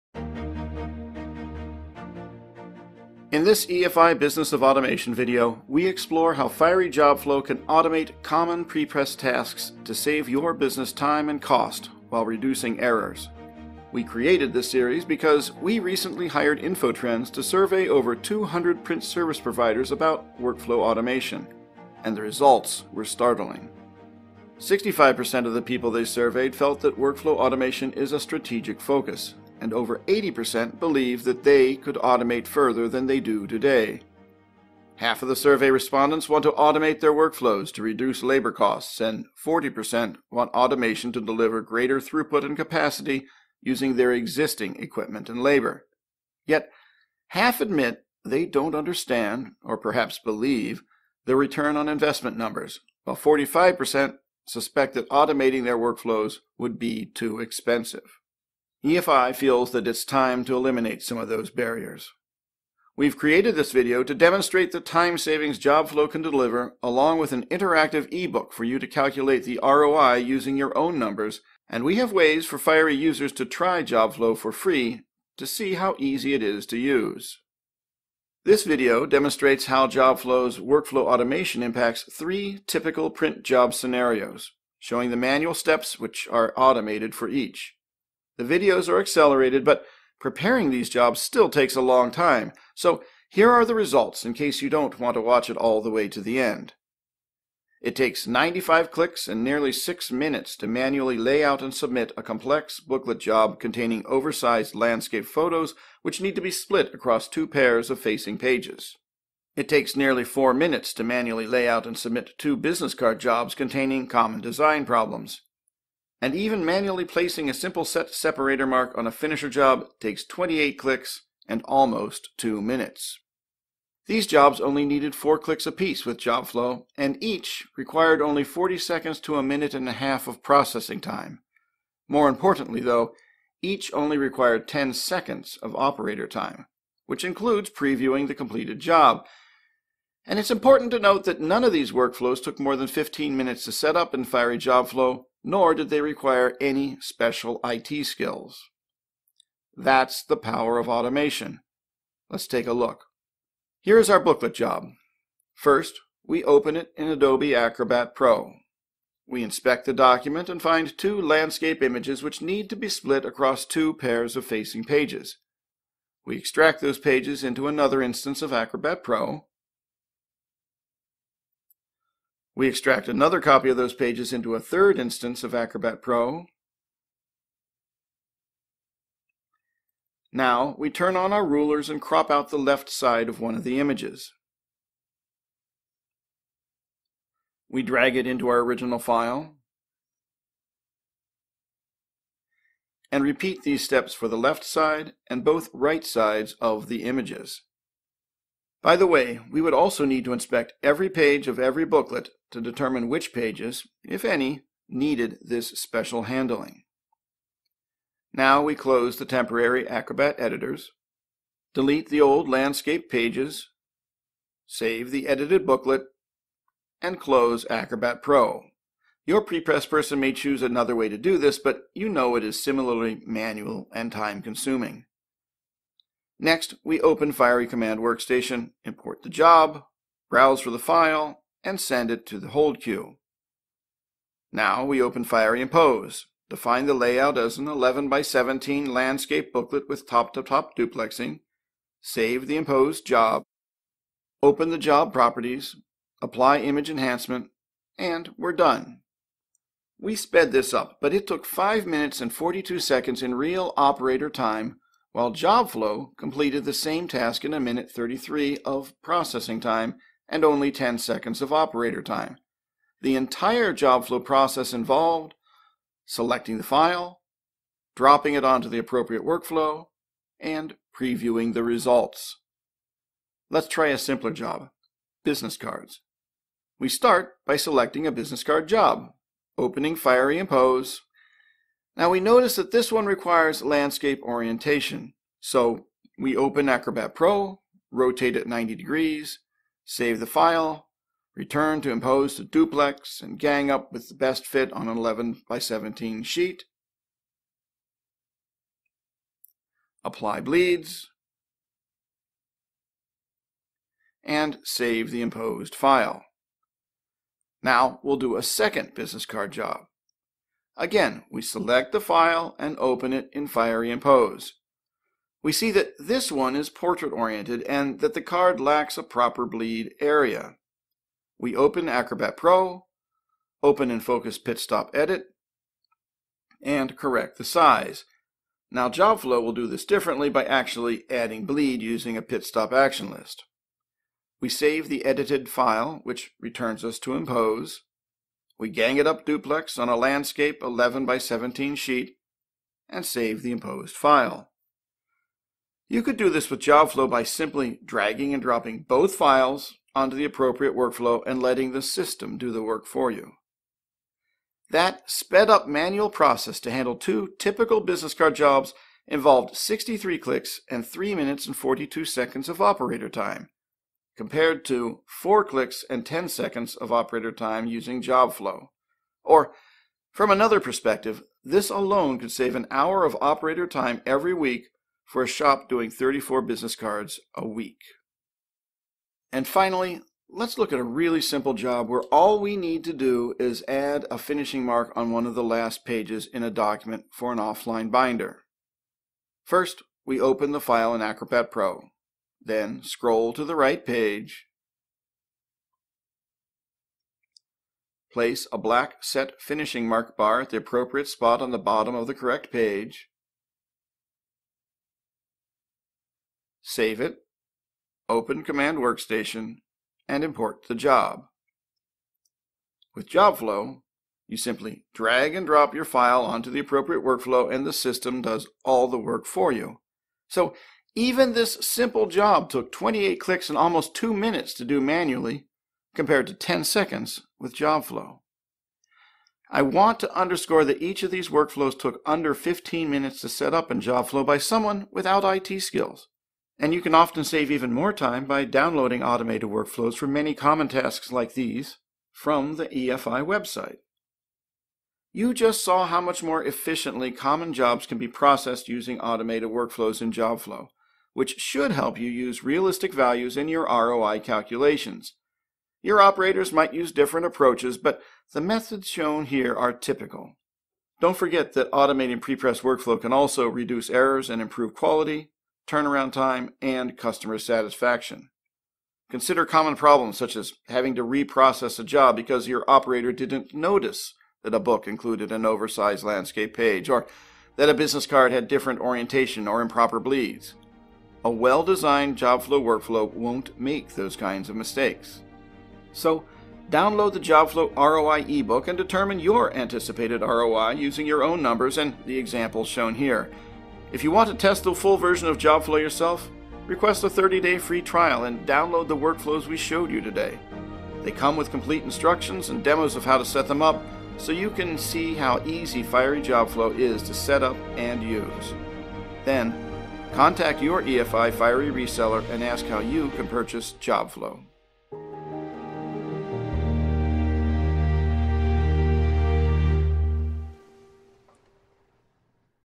The Business of Automation: Fiery JobFlow
Calculate the value of prepress automation. Fiery® JobFlow™ automates job preparation steps in prepress workflows to produce ready-to-print files. This easy-to-use print workflow automation software increases productivity and reduces costs.